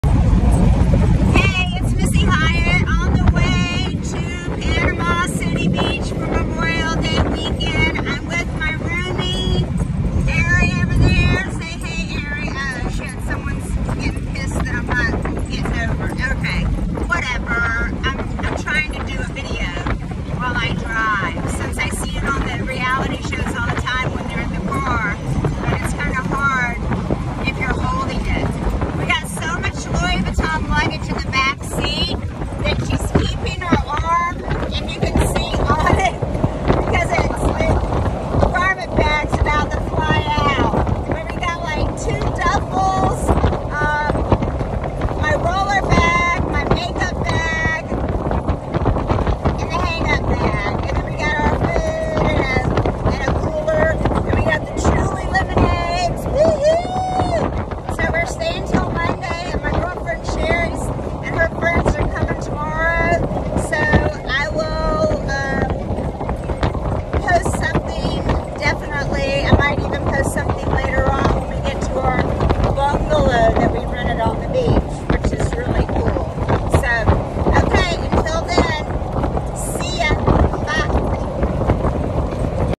Missyhyatt on the way to the redneck Riviera Beach for the weekend part one
Missyhyatt Beauty overtime